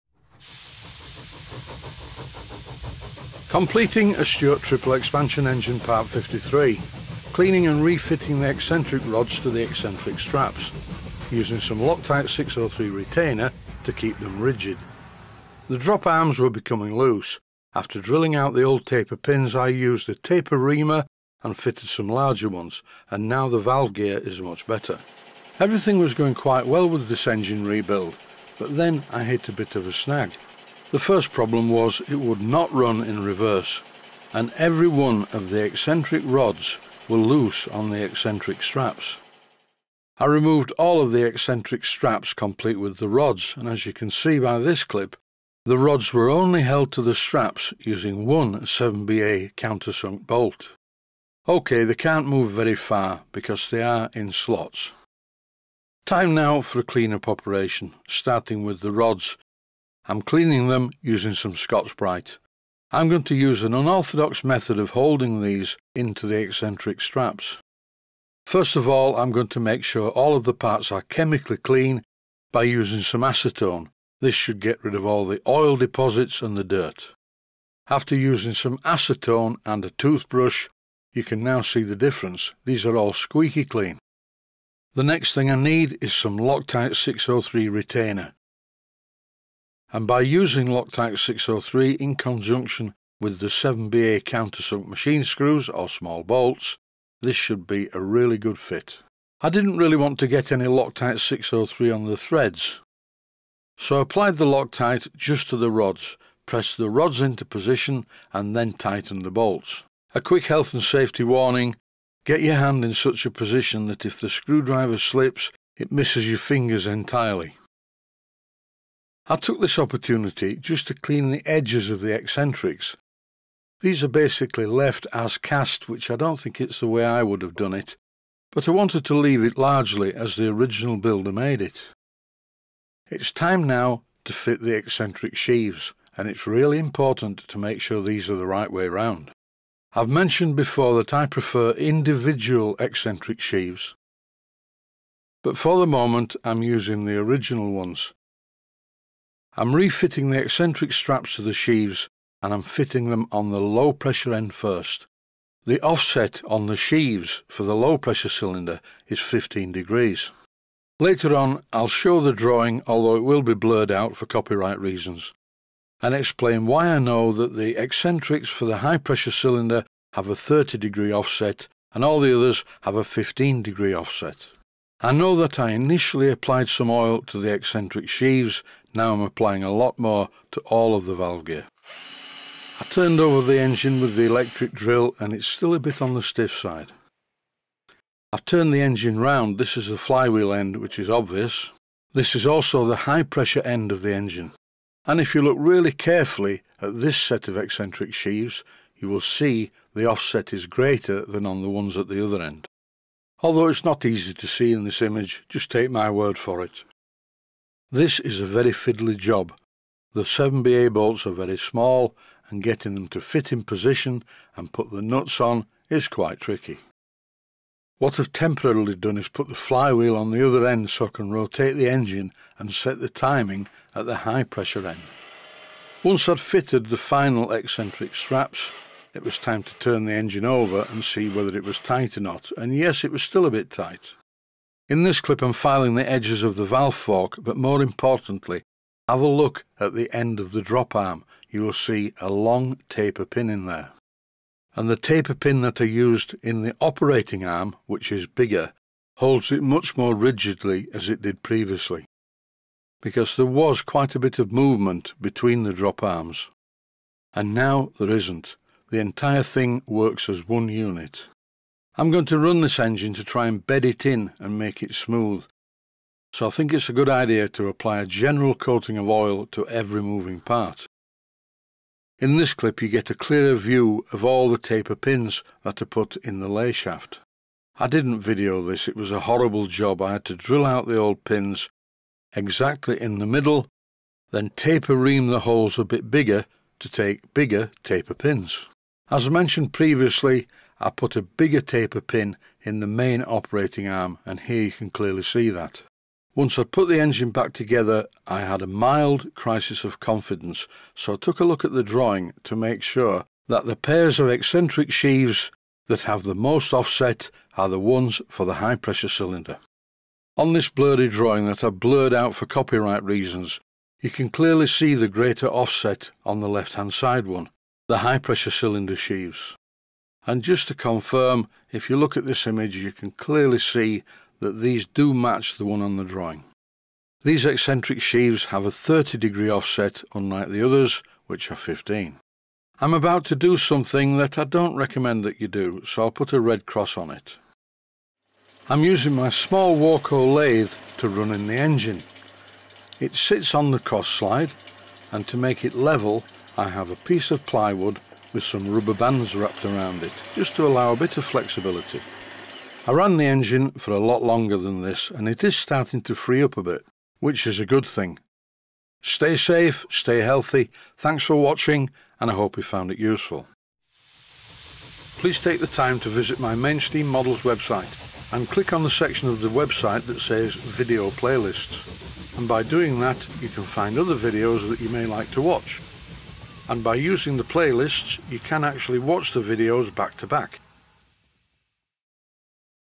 COMPLETING A STUART TRIPLE EXPANSION ENGINE - PART #53
Completing A Stuart Triple Expansion Engine - Part #53. Cleaning and refitting the Eccentric Rods to the Eccentric Straps using some Loctite 603 Retainer to keep them rigid. The drop arms were becoming loose. After drilling out the old Taper Pins, I used a Taper Reamer and fitted some larger ones and now the valve gear is much better.